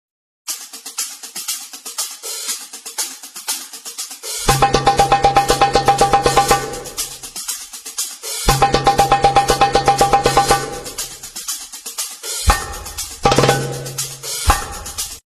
Black Labrador loves to play with his Drums 🥰
Look at him producing music. The new David Guetta

Follow WhatDaDogDoin? for more doggo shorts 🌠